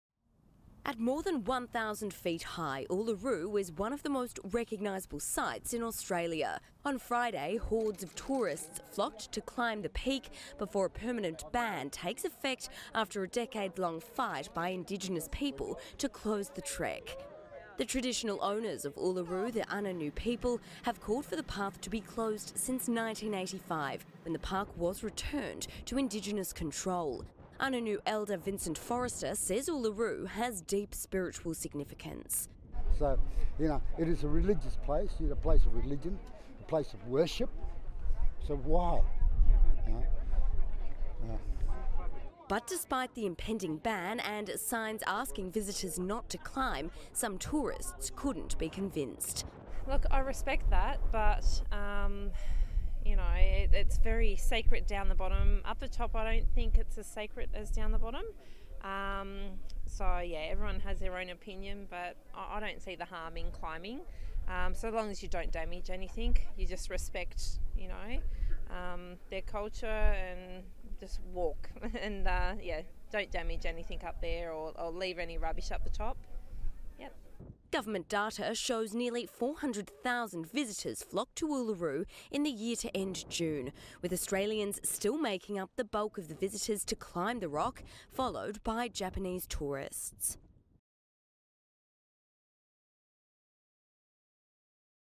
Tourists scramble to Uluru ahead of climbing ban
Hundreds of tourists formed long queues to climb Australia's Uluru soon after dawn on Friday, the day before a permanent ban on the climb takes effect following a decades-long fight by indigenous people to close the trek.